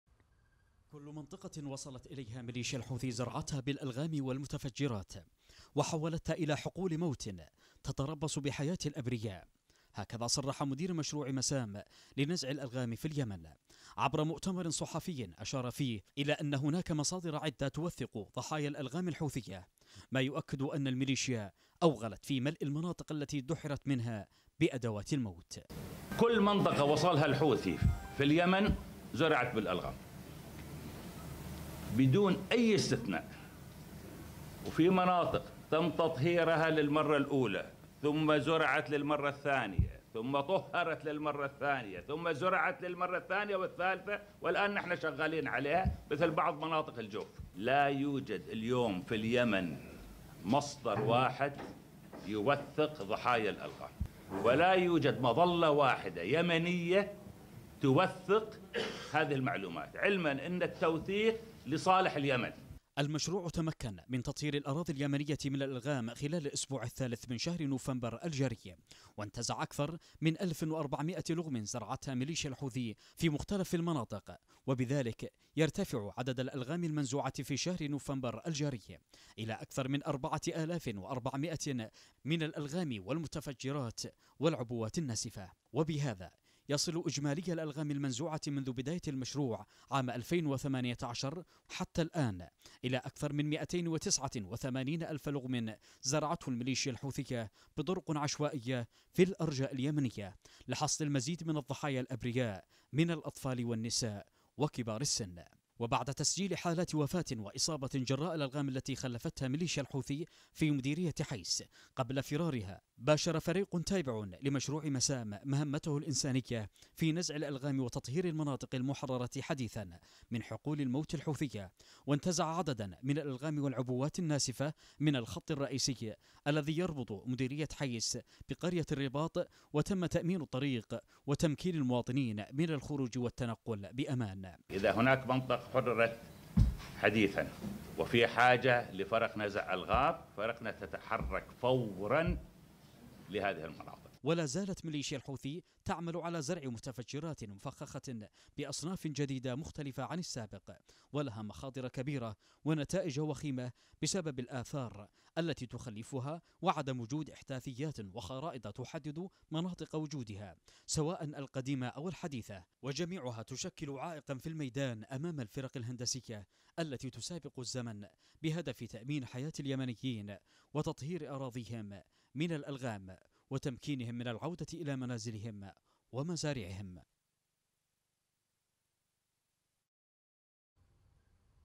الفرق الهندسية تواصل جهودها لتطهير المناطق التي ملأتها المليشيات بالألغام
تواصل الفرق الهندسية جهودها لتطهير المناطق التي ملأتها ميليشيا الحوثي بالمتفجرات والألغام .. حيث طهرت الفرق التابعة للقوات المشتركة مناطق زرعتها ميليشيا الحوثية بالألغام والعبوات المتفجرة في القرى والضواحي الشمالية بمدينة حيس التي تم تحريرها من الحوثي  خلال اليومين الماضيين ومدير مشروع مسام يؤكد ان أي منطقة جغرافية تصل إليها الميليشيا المدعومة من إيران تزرعها بالألغام

تليجرام